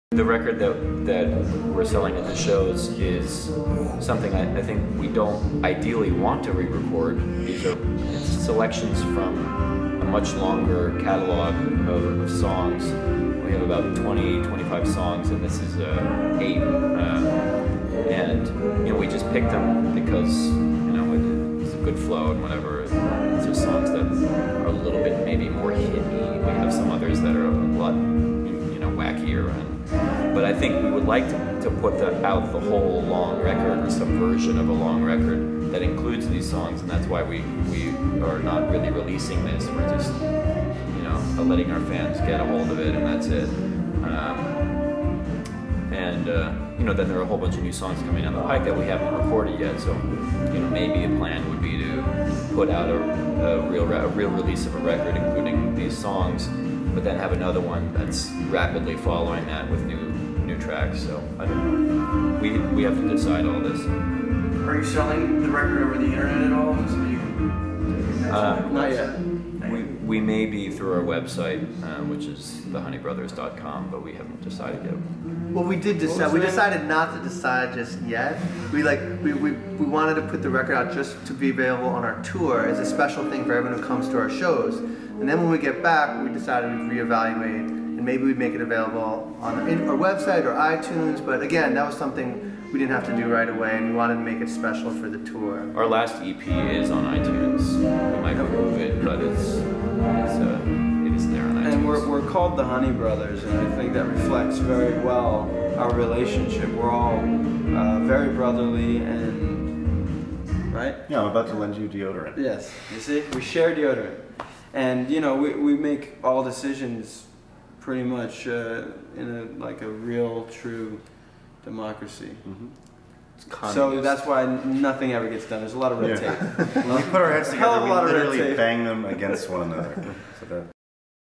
The Honey Brothers on The Record
- The Honey Brothers, a band from New York City, talk about their record. They explain their reasons for not wanting to re-record the songs from their record. The band also shares their hopes for a longer record in the future. Also in this segment, The Honey Brothers cover why they are not selling their music over the Internet. The process in which they make decisions is covered as well.